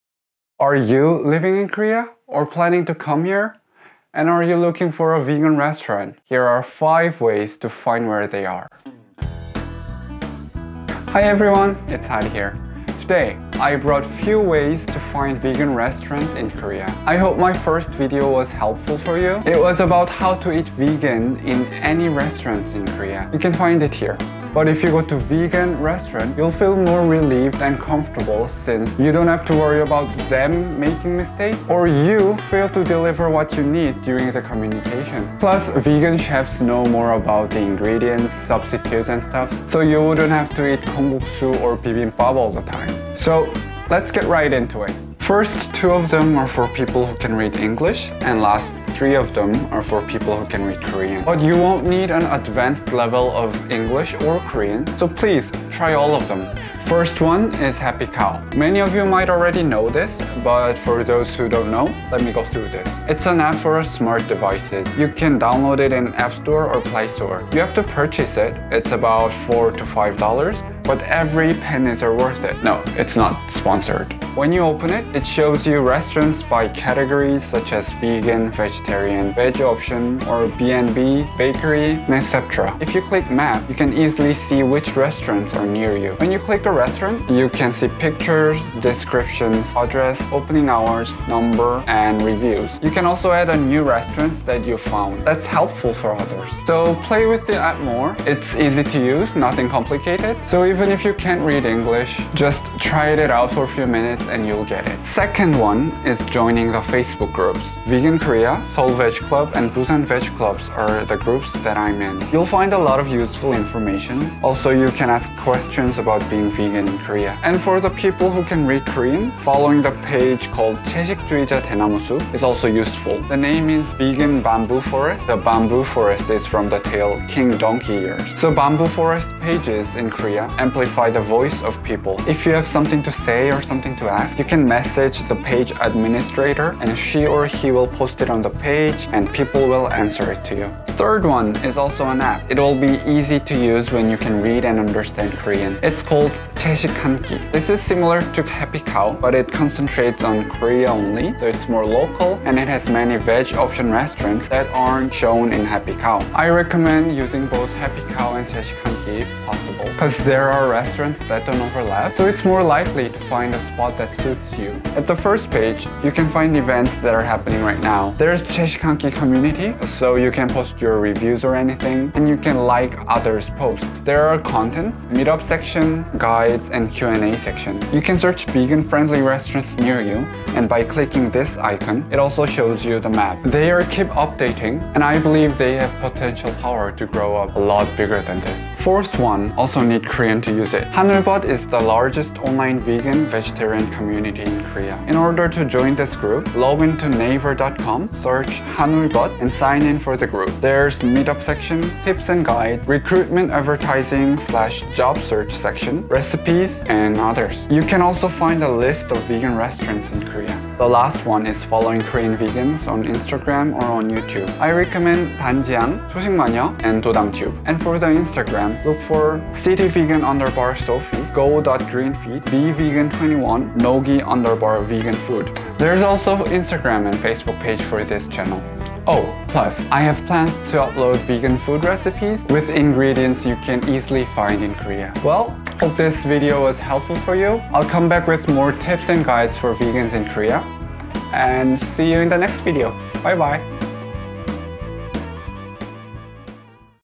ep.2 How to find vegan restaurants in South Korea! 비건 식당 찾는 법!
[한국어는 밑에]
[This video can be made with help of Korea Safety Health Environment Foundation]

*Subtitle available*

I brought five ways to find vegan restaurants in Korea!

I hope these tips are useful for you :)

Apps:
-HappyCow
-채식한끼(BeyondNext)

Facebook Groups:
-Vegan Korea
-Seoul Veggie Club
-Busan Veggie Club
-채식주의자 대나무숲

*자막 지원*

비건 음식 찾는 5가지 방법을 가지고 왔어요!

도움이 많이 되는 팁이었으면 하네요 :)

감사합니다!

페이스북: @hari.lives.here.official

앱:
-HappyCow
-채식한끼(BeyondNext)

페이스북 그룹:
-Vegan Korea
-Seoul Veggie Club
-Busan Veggie Club